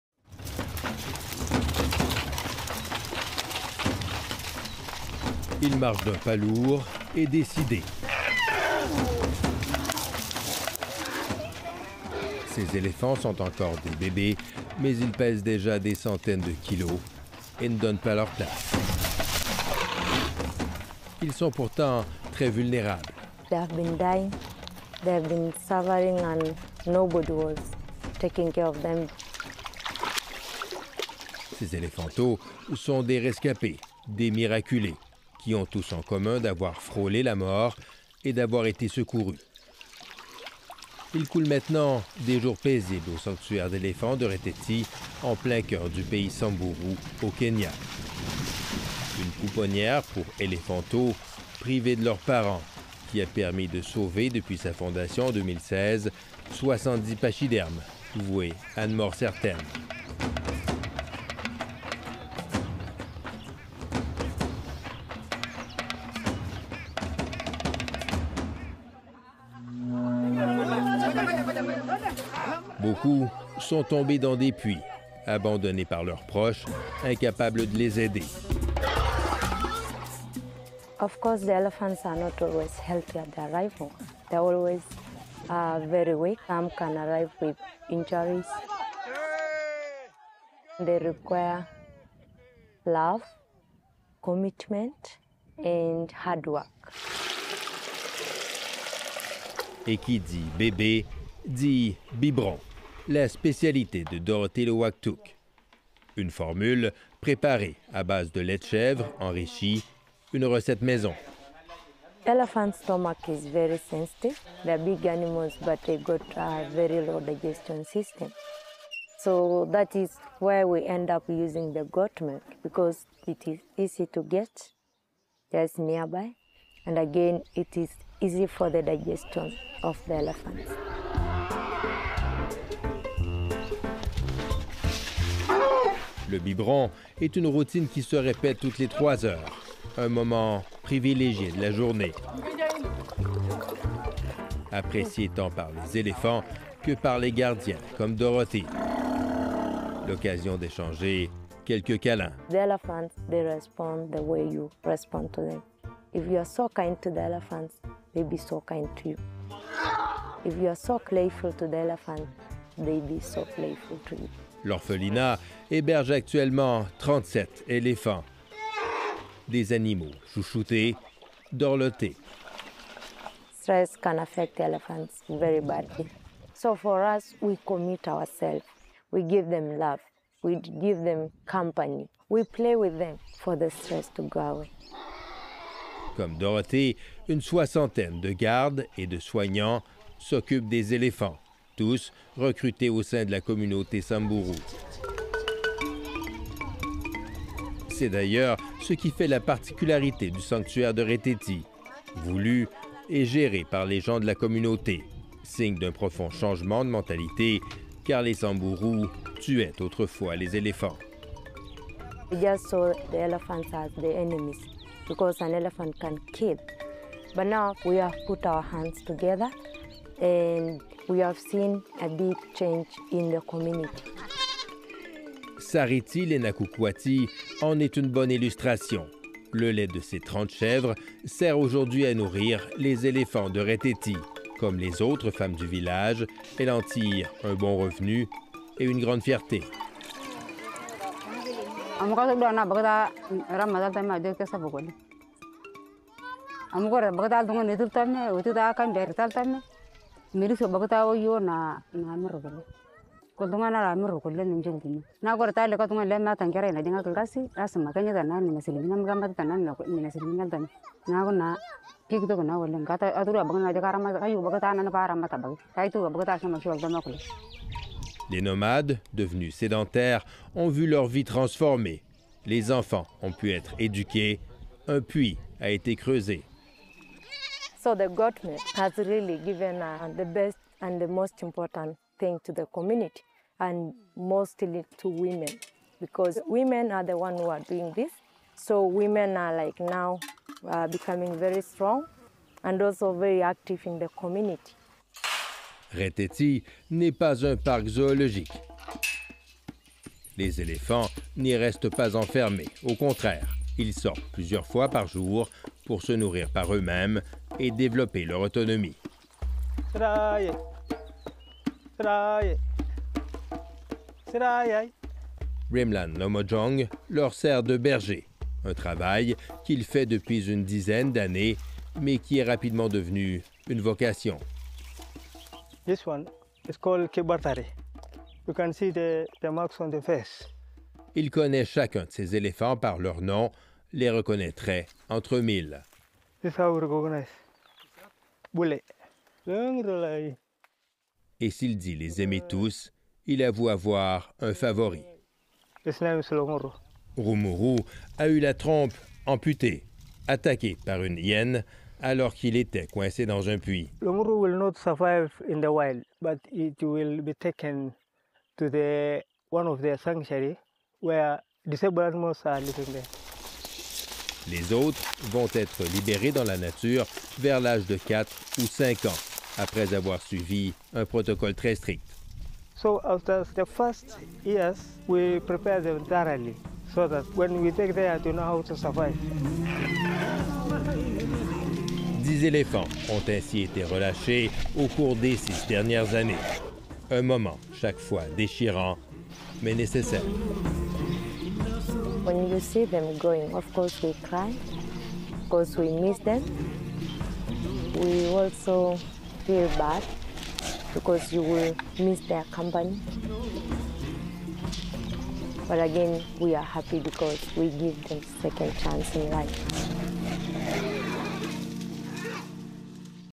Afrique : un orphelinat à la rescousse des éléphants du Kenya
Le Kenya a réussi à protéger les populations d'éléphants en s'attaquant au braconnage, mais ils restent vulnérables à la sécheresse et à leurs prédateurs.

Jean-François Bélanger nous fait découvrir un orphelinat pas comme les autres, qui donne une deuxième chance à des éléphanteaux et à toute une communauté.